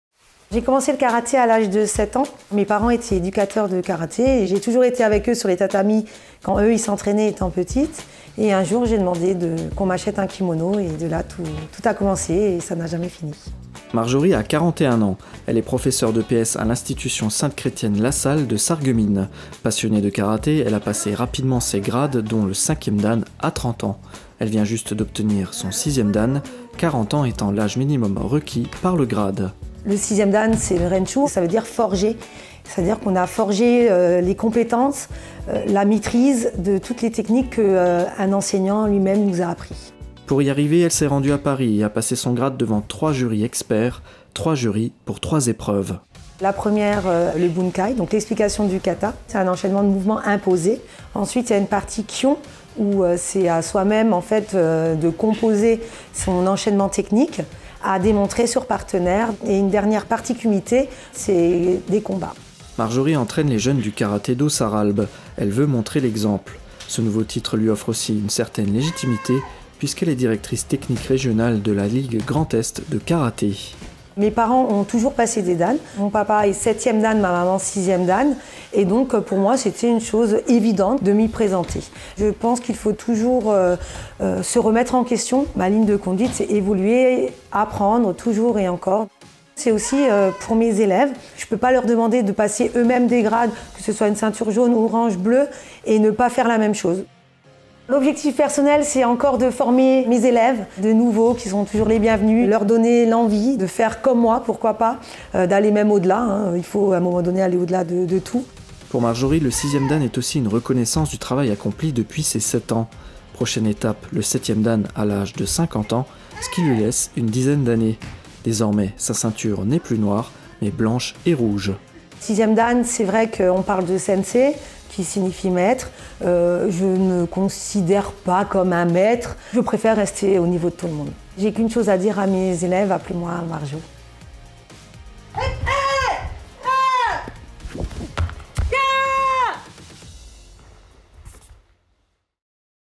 Karaté : Marjorie Lock-Carbiner est 6e dan
C’est une histoire de famille : Marjorie Lock-Carbiner, passionnée de karaté, vient d’obtenir son 6e dan. La voilà sensei.
Réalisation : David HORÈS